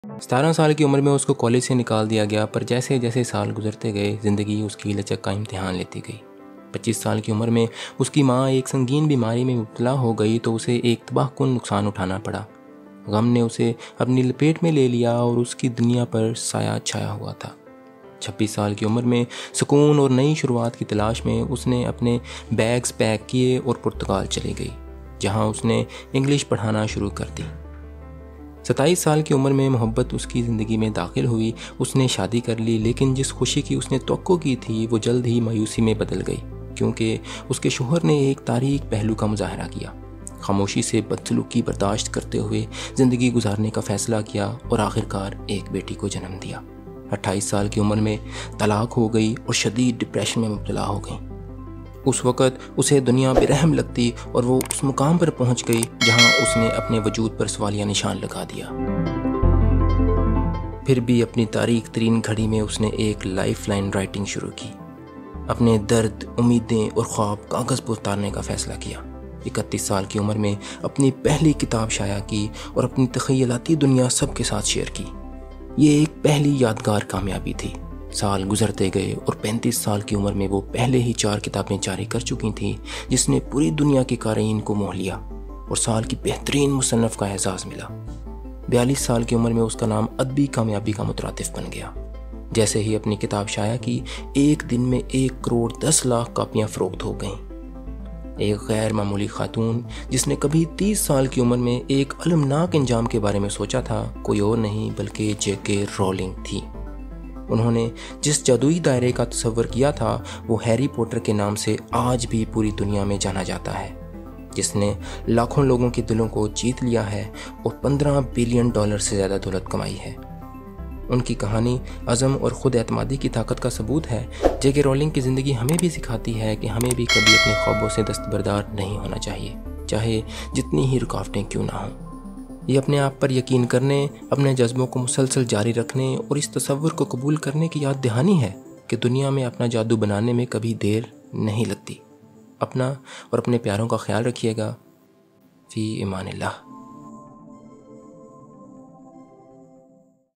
Journey of J.K. Rowling Motivation Story | LibLab | Urdu | Hindi | اردو | हिंदी
Step into the captivating world of J.K. Rowling as we unravel her remarkable life story. From the sizzling success of Harry Potter to the controversies that have surrounded her, J.K. Rowling's journey is a testament to her resilience and inspiration.
Embark on a spellbinding documentary that takes you on a behind-the-scenes tour of J.K. Rowling's life. Witness the triumphs, the challenges, and the cancelled moments that have defined her path. Get an exclusive glimpse into her thoughts and experiences through a candid interview that tackles the headlines head-on.
Discover the enigmatic story of J.K. Rowling, where she defied the odds, faced controversy, and emerged as an inspirational figure. Join us on this unforgettable journey through the life, words, and legacy of J.K. Rowling.